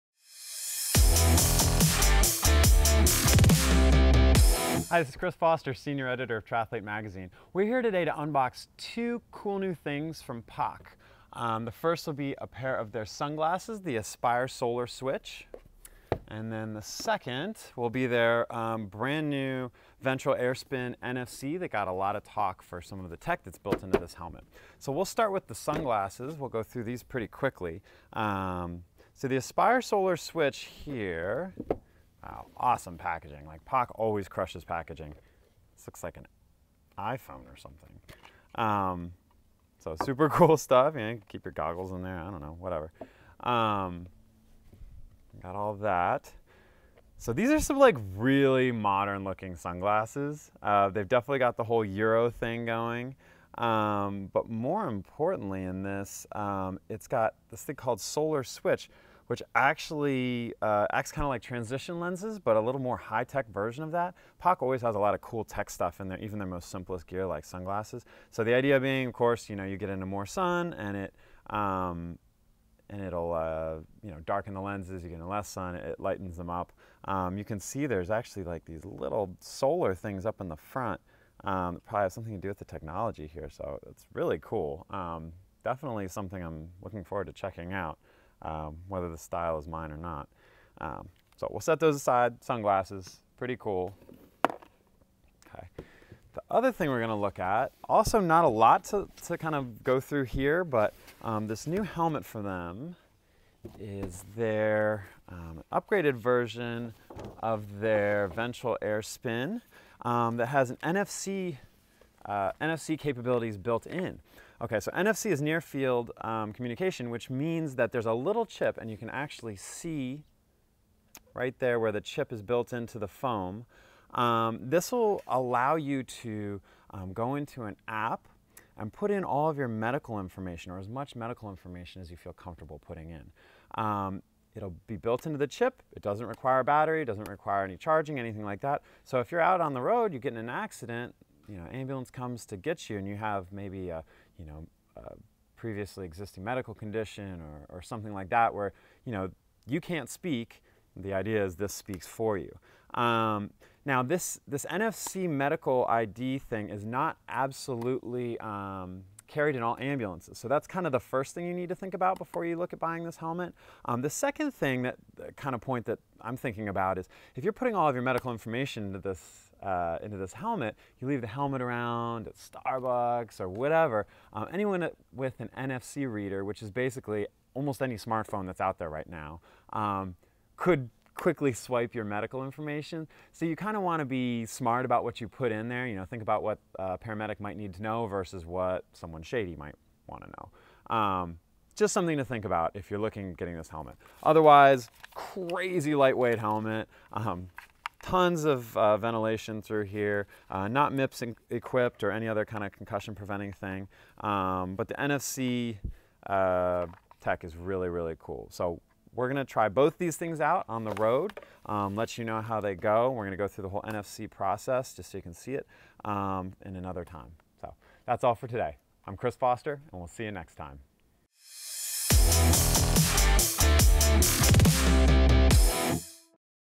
Unboxed: Crazy New Tech From POC
Senior editor Chris Foster unboxes some crazy new tech from POC—the new NFC-equipped Ventral Air NFC SPIN helmet and the Aspire Solar Switches sunglasses.

The Ventral helmet includes a built in NFC chip that houses your medical information to be retrieved wirelessly by medical personnel; the Aspire Solar Switch changes from light to dark in an instant via built-in sensors.